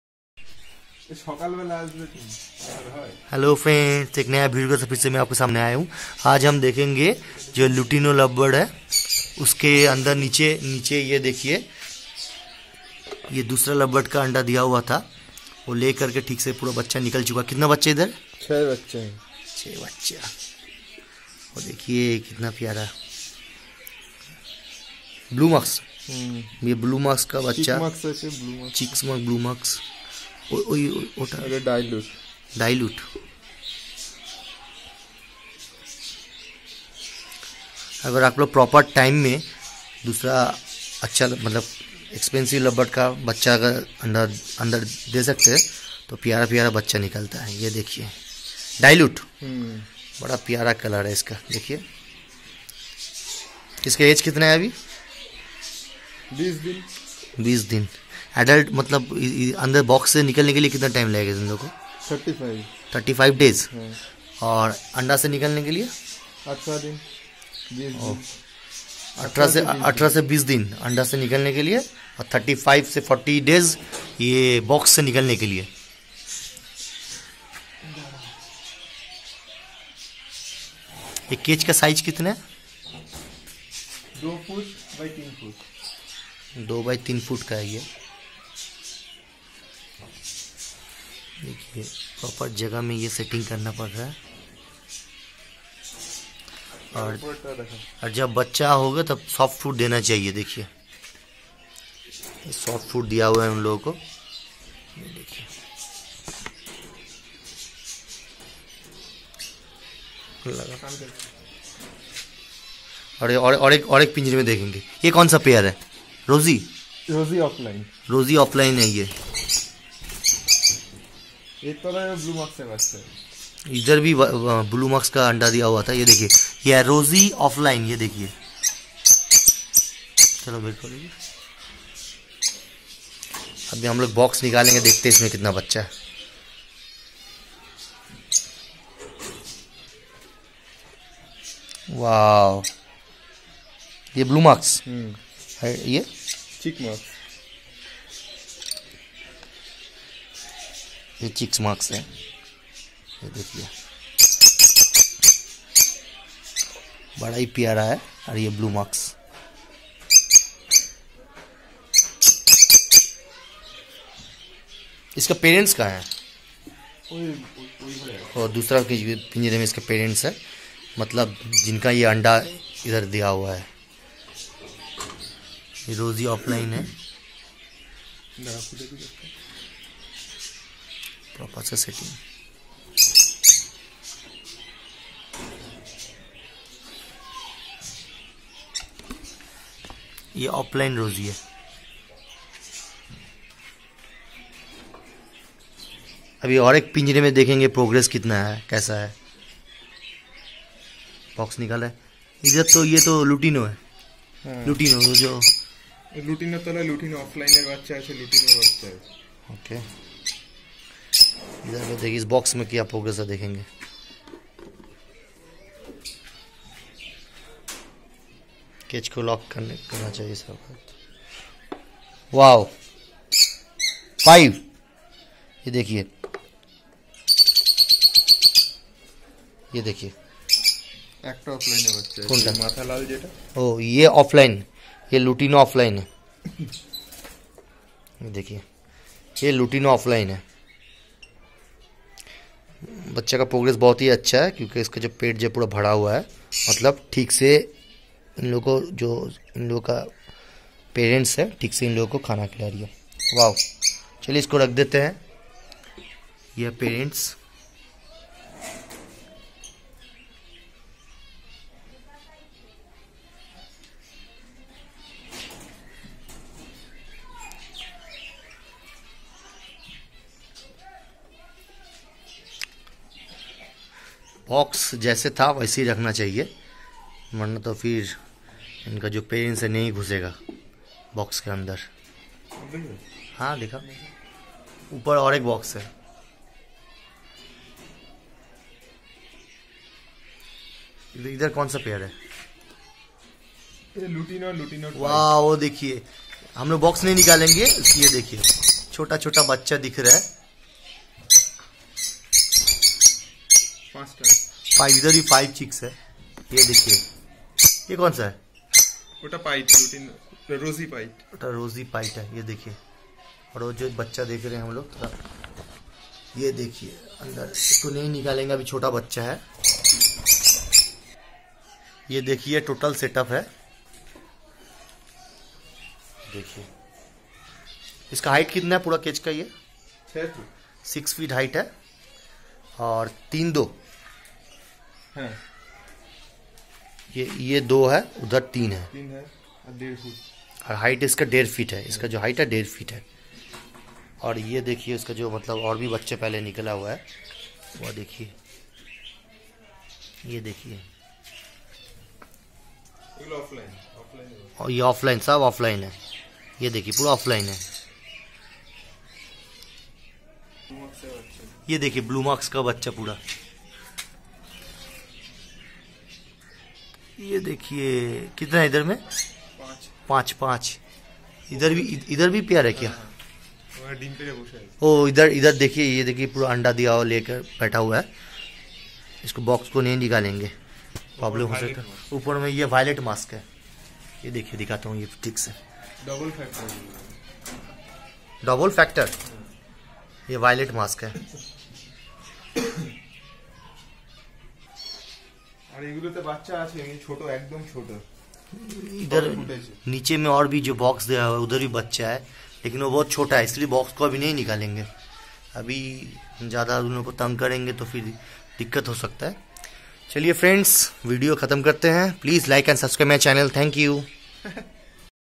Love bird breeding progress,egg exchange and setup view
in this video I showed  Lovebird setup and also their breeding progress and how u can exchange eggs with other love birds breeds